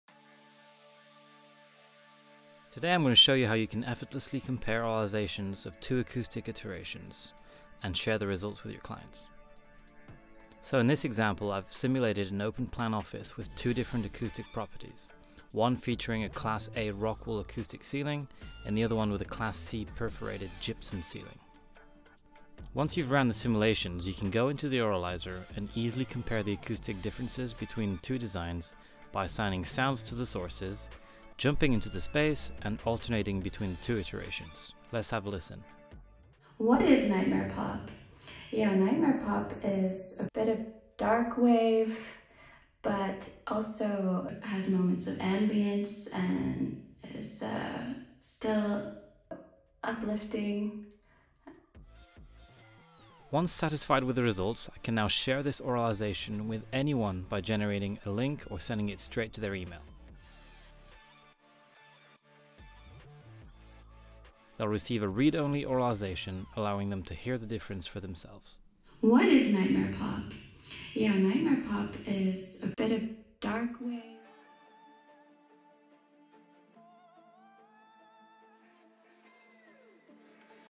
Acoustic Comparisons in Treble Auralizer
Effortlessly compare auralizations of various acoustic iterations and share the results seamlessly with your clients.

In this video, we dive into a practical example: an open-plan office with two distinct acoustic properties – a Class A rockwool acoustic ceiling and a Class C perforated gypsum ceiling. See how easy it is to compare and share auralizations via a generated link or directly through email.